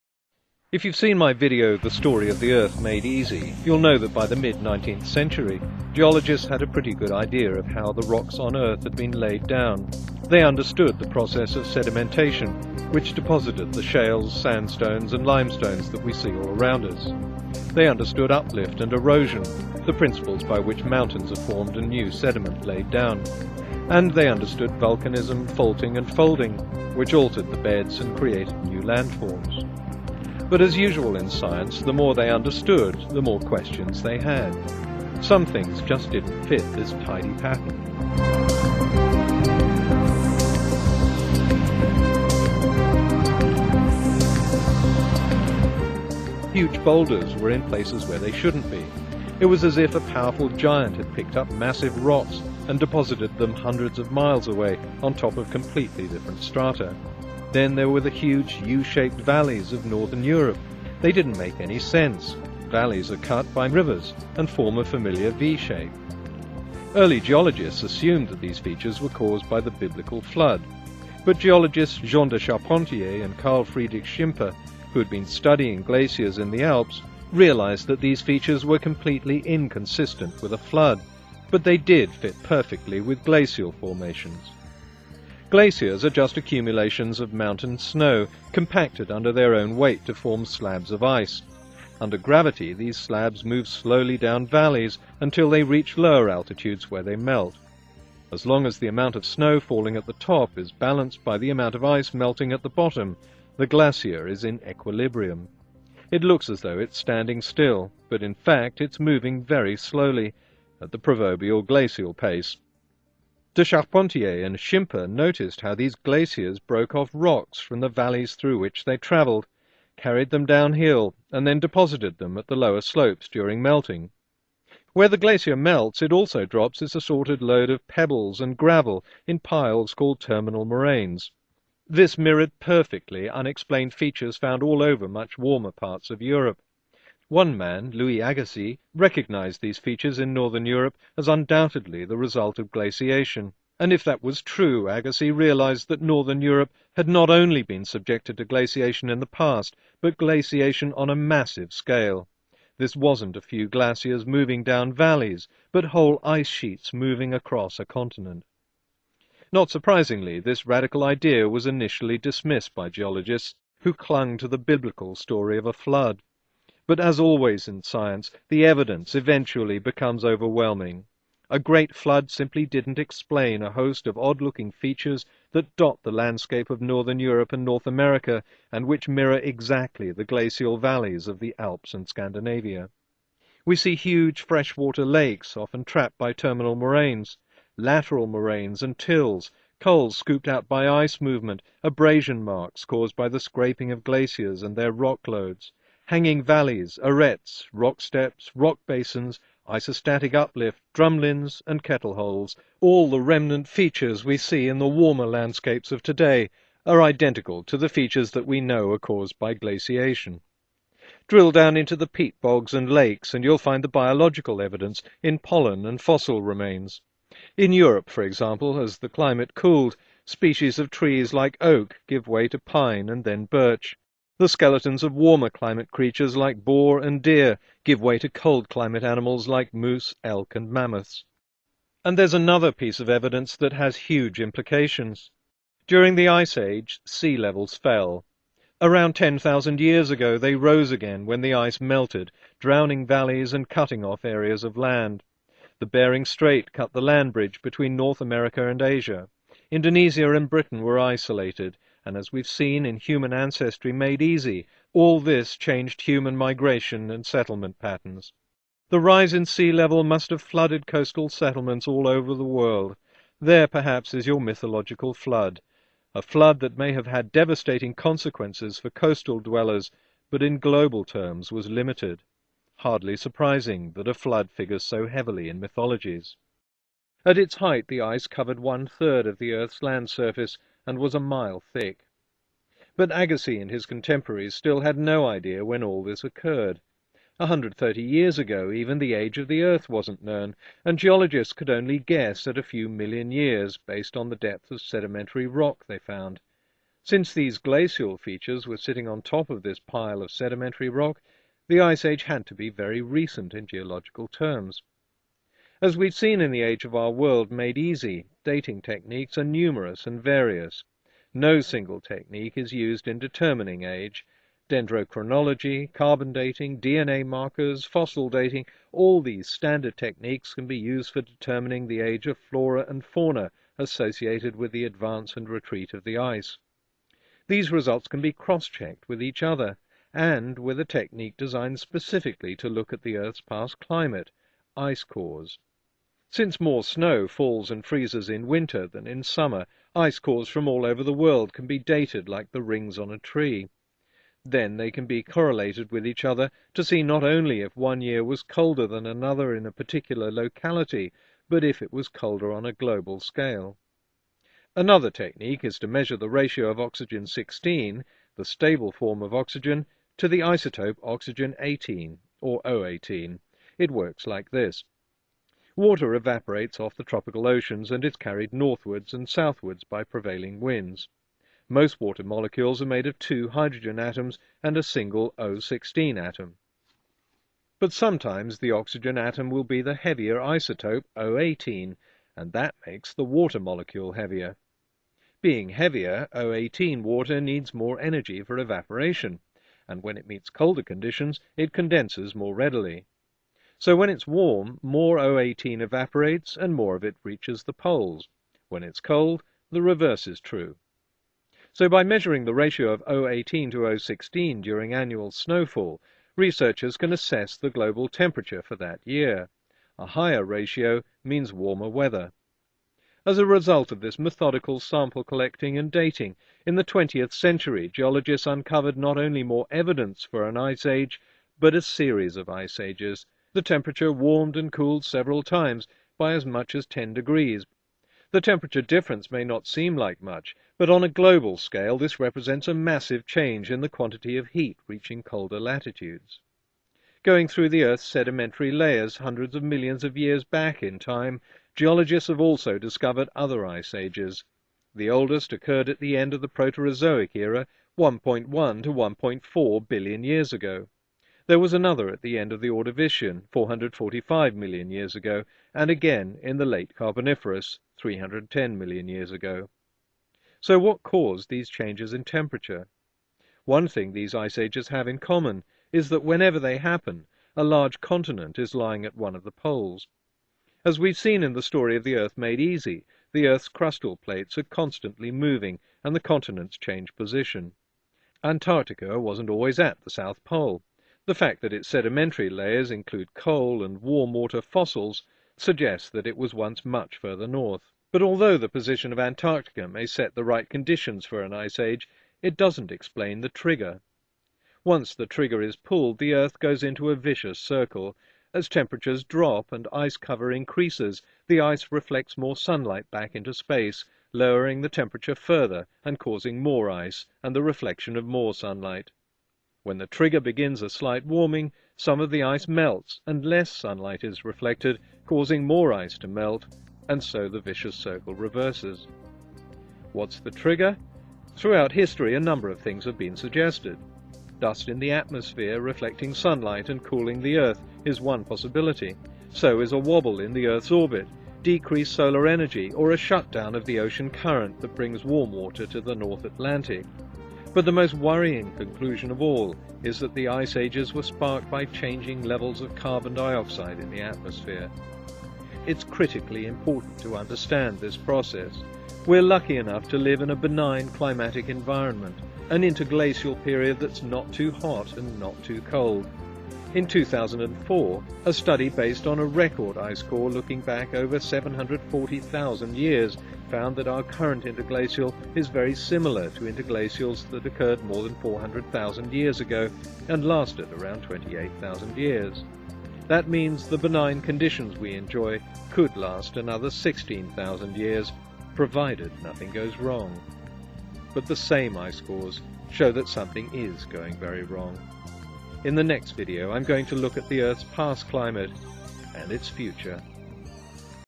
Potholer54: "The Ice Age"
What was the Ice Age? How do we know it happened? And will it happen again?
This video should by rights be called "The Ice Age Made Easy", but I want to reserve that title for the series of videos I made showing the evidence for our origins. But the principle is the same -- what happened and how do we know.

CORRECTION: At 5:55 I called O-16 the "stable" form of oxygen. But in fact, both O-16 and O-18 are stable. I should have said O-16 is the most abundant.